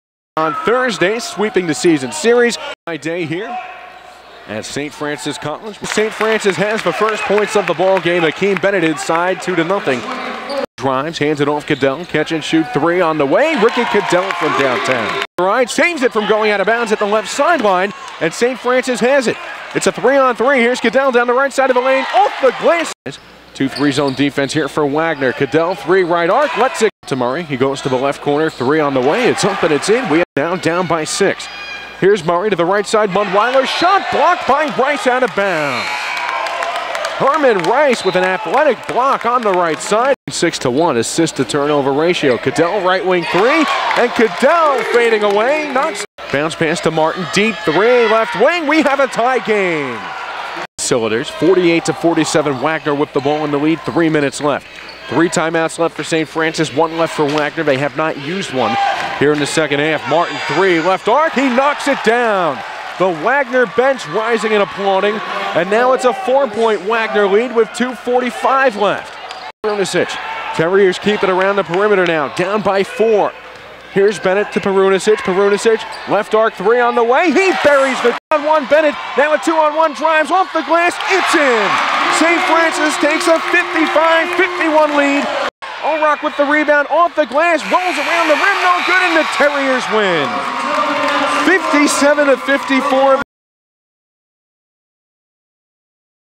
The St. Francis Mens Basketball Team Wins 57-54 Against The Wagner College Seahawks
Seth Cantor Delivers the Play-By-Play As Brian Nash's St. Francis Terriers beat North East Conference Rival Wagner College on Saturday, January 23rd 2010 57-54 for the Terrier Men's Second Consecutive Win.